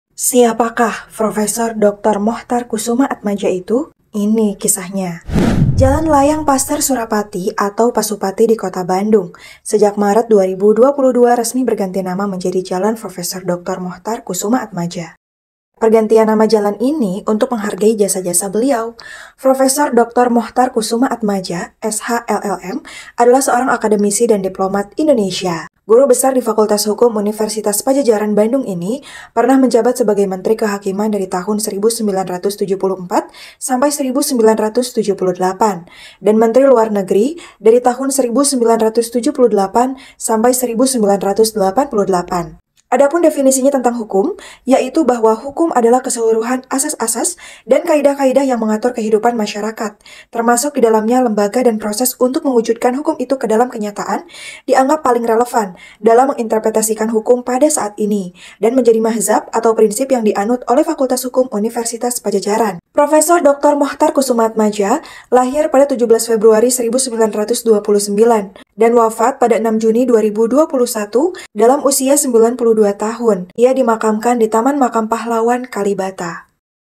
Siapakah Prof Dr. Mochtar Kusumaatmadja Itu?
Siapakah Prof Dr. Mochtar Kusumaatmadja Itu? Ini Kisahnya

Video & VO by @tiwikasavela